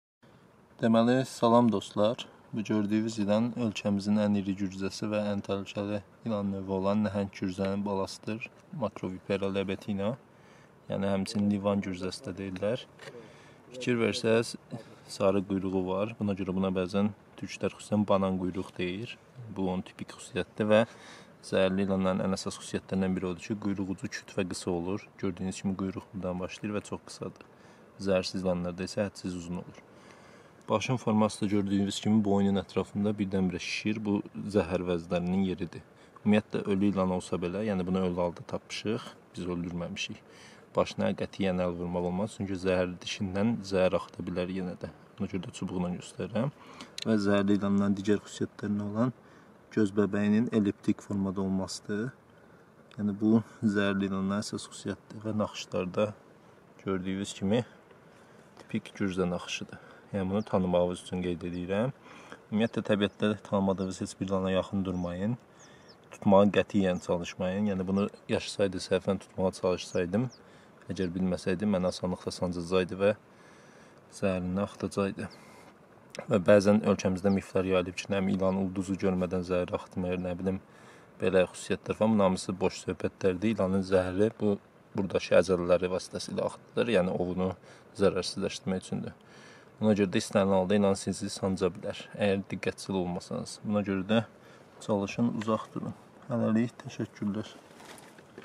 Gürzə balası - Nəhəng gürzə ilanı haqqında - Cənubi Qafqaz gürzəsi -lat. Macrovipera lebetina obtusa
Gürzələr fəsiləsinin ən iri nümayəndəsidir. Onun quyruğu ilə birlikdə uzunluğu 2 metr, çəkisi isə 3 kq təşkil edir. Başı olduqca iri və enlidir. Ağzı yuvarlaq, gözləri şaqulidir. Yuxarı hissəsi boz-qəhvəyi tondadır. Bəziləri vardır ki, bütünlüklə qara və qəhvəyi rəngdə olur.